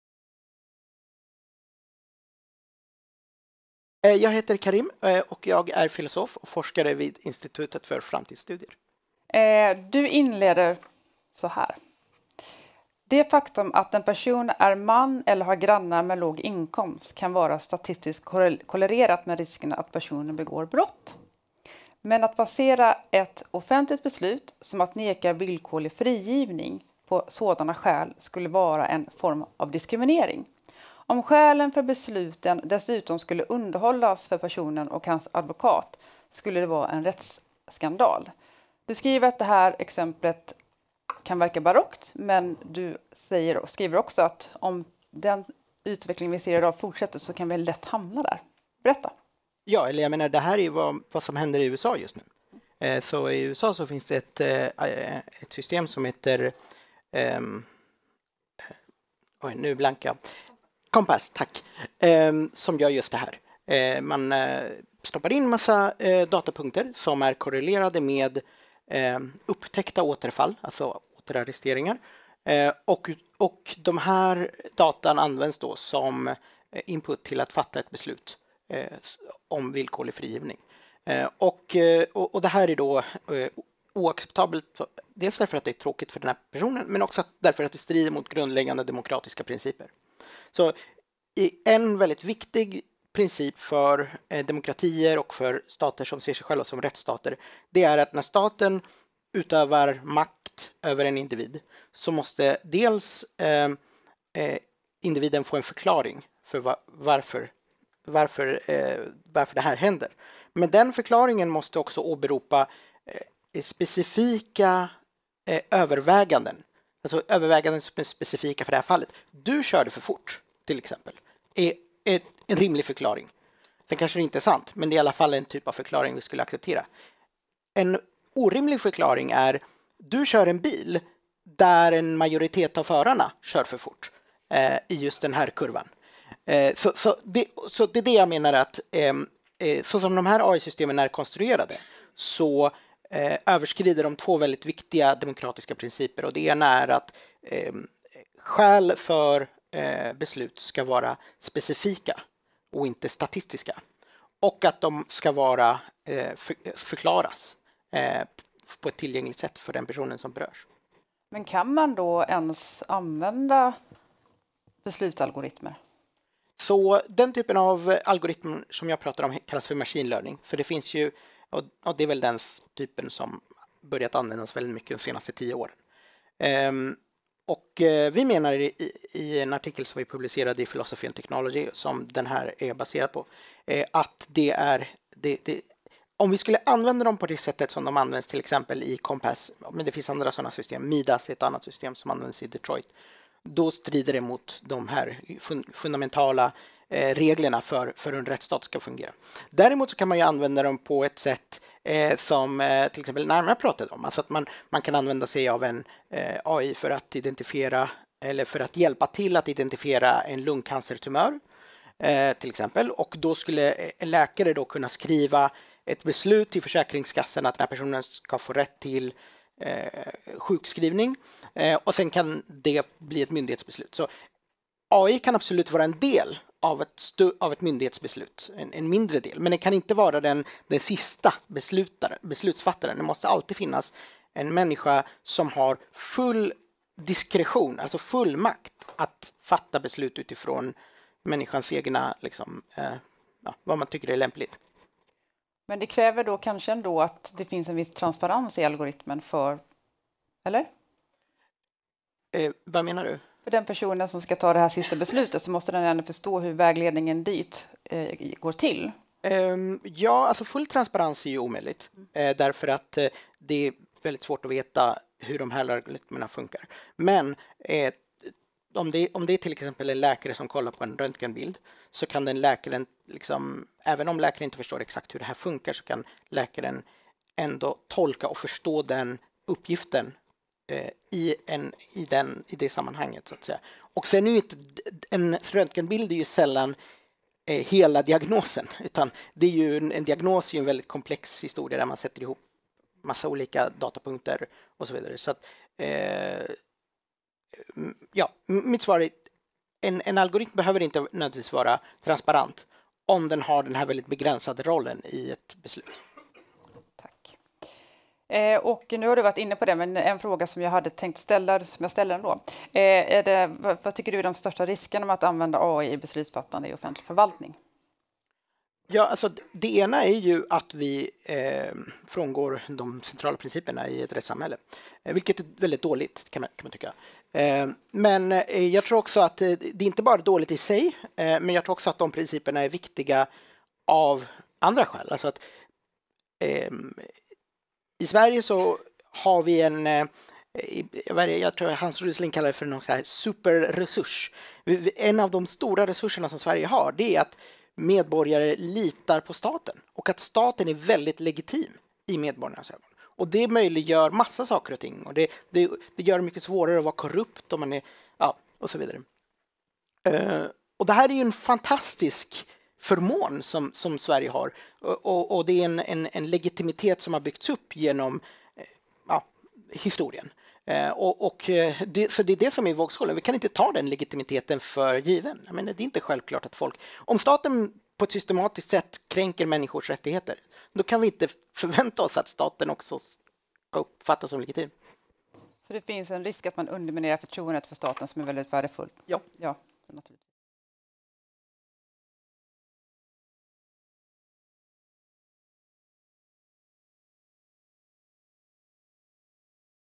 Artificiell intelligens och offentlig rättfärdighet
Karim Jebari talar under lanseringen av antologin "AI & makten över besluten" hos Internetstiftelsen.